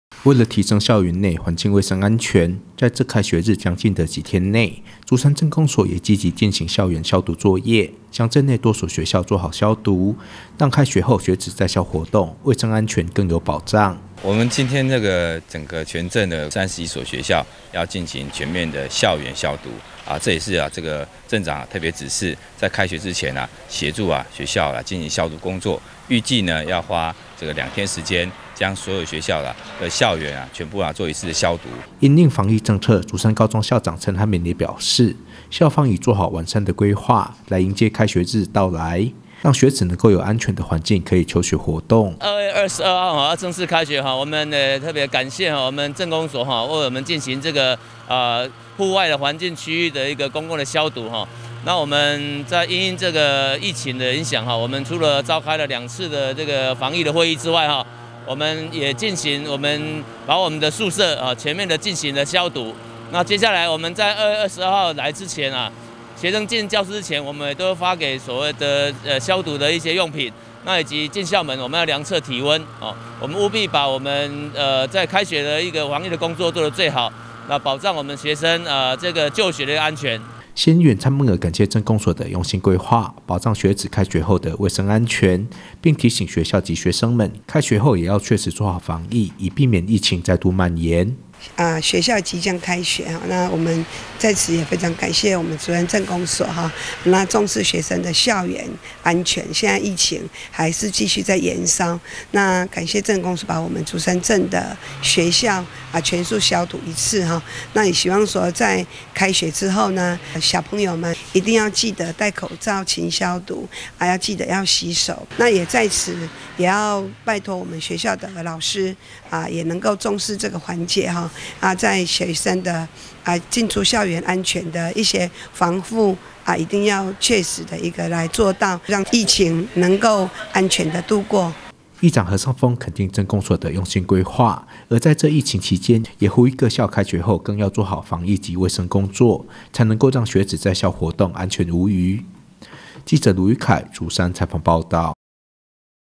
20210220 民議新聞 開學日在即 鎮公所積極消毒保障學子在校衛生安全(縣議員 蔡孟娥)
開學日將近，為了提升校園內環境衛生安全，近幾日竹山鎮公所也積極進行校園消毒作業，將鎮內多所學校做好消毒，盼開學日來臨時，讓學子在校活動，衛生安全更有保障。議長何勝豐肯定鎮公所的用心規劃，而在這疫情期間，也呼籲各校開學後更要做好防疫及衛生工作，才能夠讓學子在校活動安全無虞。